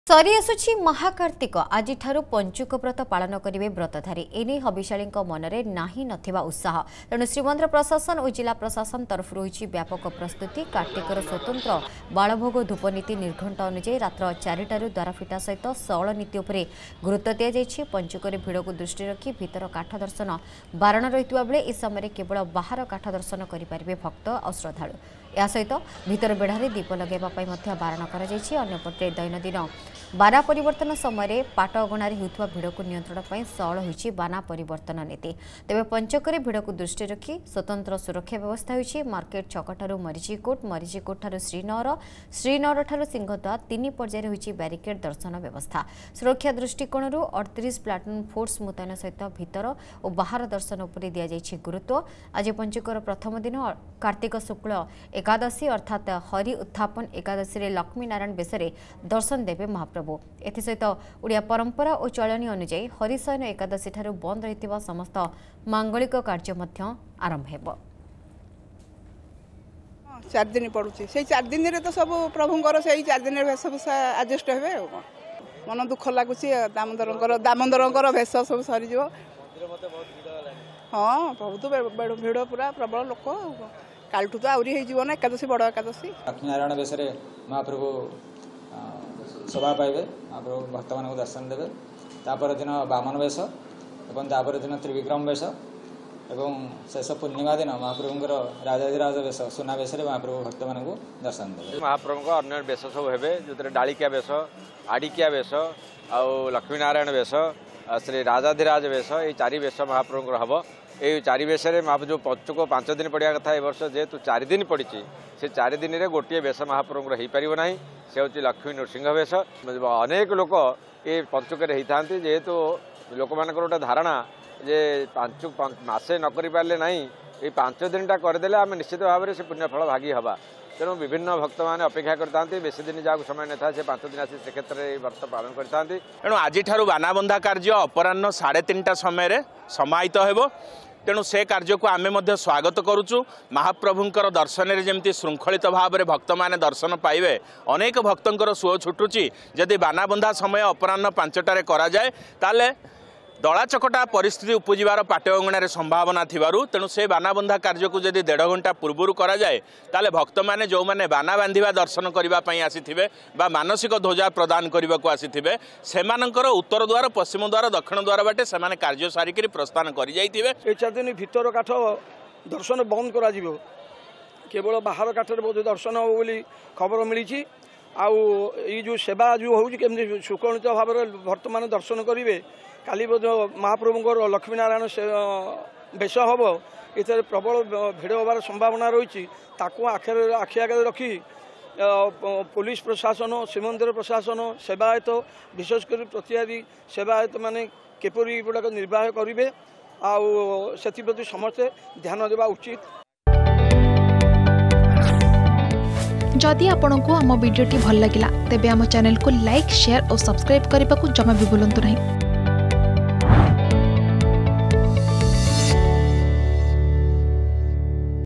ଆଜିଠାରୁ ପଞ୍ଚୁକବ୍ରତ ପାଳନ କରିବେ ବ୍ରତଧାରୀ ।|| Puri Kartika Month
ଆଜିଠାରୁ ପଞ୍ଚୁକବ୍ରତ ପାଳନ କରିବେ ବ୍ରତଧାରୀ । ଏନେଇ ହବିଷ୍ୟଳୀଙ୍କ ମନରେ ନାହିଁ ନଥିବା ଉତ୍ସାହ । ତେଣୁ ଶ୍ରୀମନ୍ଦିର ପ୍ରଶାସନ ଓ ଜିଲ୍ଲା ପ୍ରଶାସନ ତରଫରୁ ହୋଇଛି ବ୍ୟାପକ ପ୍ରସ୍ତୁତି ।